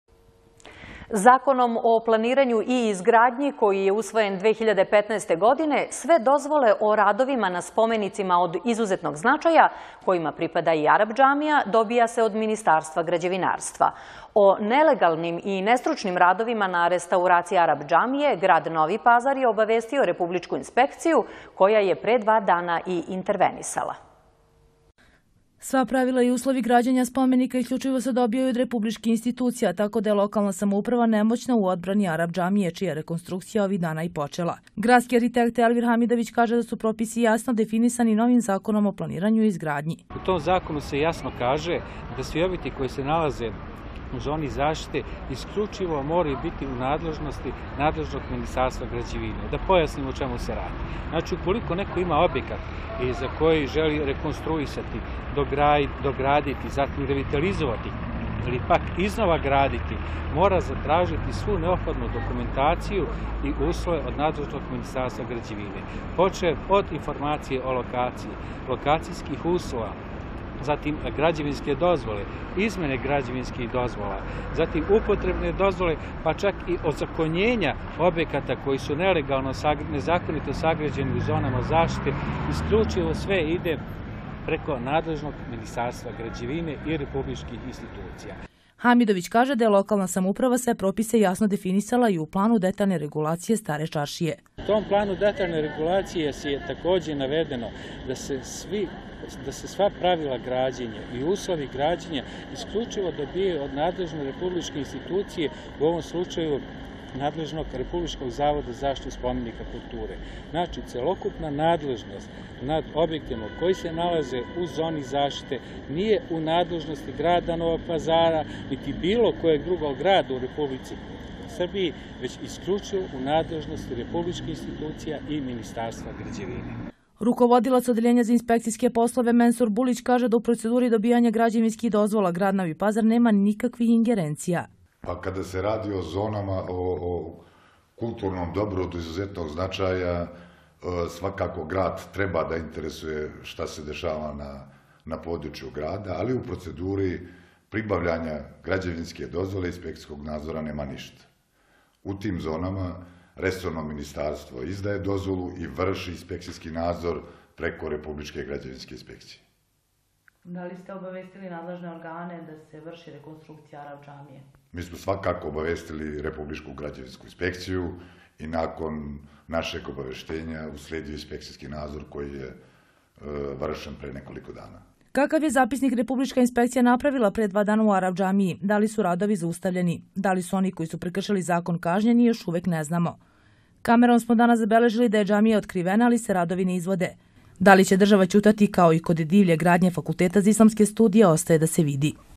Lokalna samouprava nemoćna u odbrani spomenika
Zakonom o planiranju i izgradnji, koji je usvojen 2015.godine sve dozvole o radovima na spomenicima od izuzetnog značaja, kojima pripada i Arap džamija dobija se od Ministatrstva građevinarstva. O nelegalnim I nestručnim radovima na restauraciji Arap džamije, grad Novi Pazar je obavestio republičku inspekciju koja je pre dva dana i intervenisala.